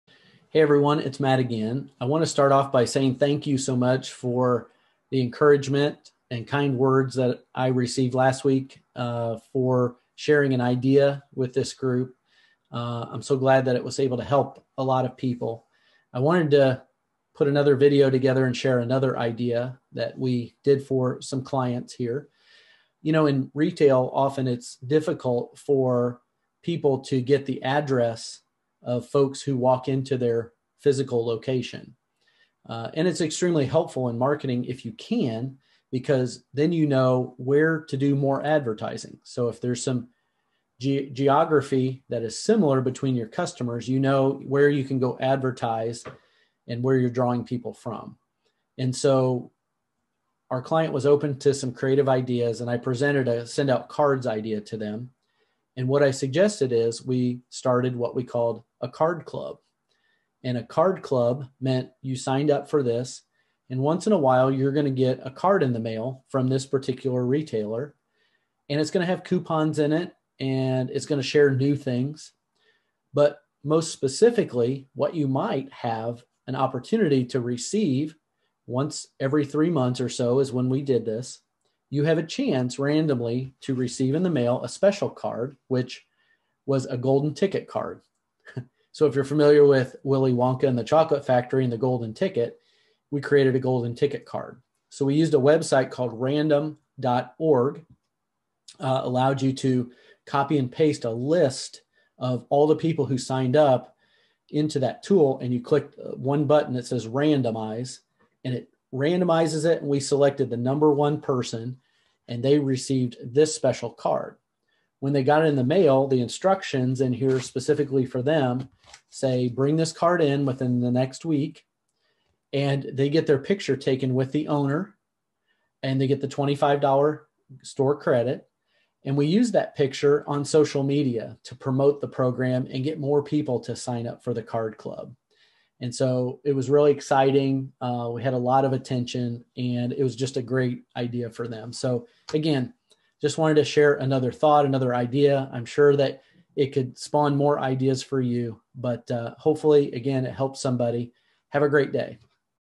Send Out Cards - Creative Idea 02
Here's a "golden" idea for those of you who have clients and prospects in retail. Hope this helps someone today!